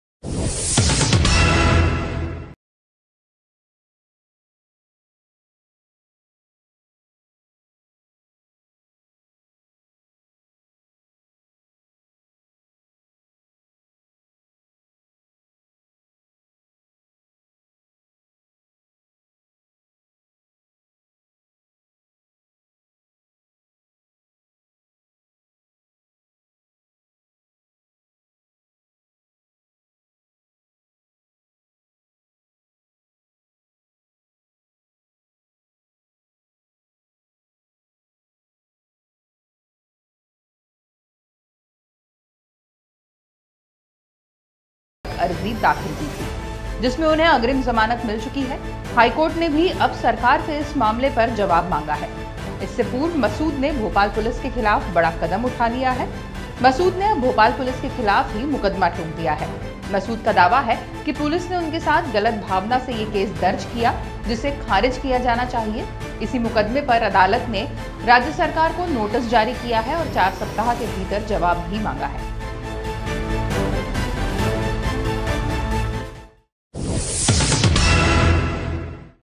Congress विधायक ने चला बड़ा दांव. सकते में पूरी Shivraj सरकार
कांग्रेस विधायक आरिफ मसूद पर मामला दर्ज कर अदालत तक घसीटना अब भोपाल पुलिस को भारी पड़ रहा है. आपको याद दिला दें बीती 30 अक्टूबर को भोपाल के इकबाल मैदान में भीड़ इकट्ठा कर मसूद ने फ्रांस के राष्ट्रपति का पुतला और झंडा जलाया था. इस घटना के बाद विधायक मसूद समेत चार सौ लोगों के खिलाफ मामला दर्ज हुआ था. जिसमें से सिर्फ आरिफ मसूद के अलावा बाकी सभी लोग अज्ञात थे. इस मामले मे आरिफ मसूद ने हाईकोर्ट तक अर्जी दाखिल की थी. जिसमें उन्हें अग्रिम जमानत मिल चुकी है. हाईकोर्ट ने भी अब सरकार से इस मामले पर जवाब मांगा है. इससे पूर्व मसूद ने भोपाल पुलिस के खिलाफ बड़ा कदम उठा लिया है. मसूद ने अब भोपाल पुलिस के खिलाफ ही मुकदमा ठोक दिया है. मसूद का दावा है कि पुलिस ने उनके साथ गलत भावना से ये केस दर्ज किया. जिसे खारिज किया जाना चाहिए. इसी मुकदमे पर अदाल नत राज्य सरकार को नोटिस जारी किया है और चार सप्ताह के भीतर जवाब मांगा है.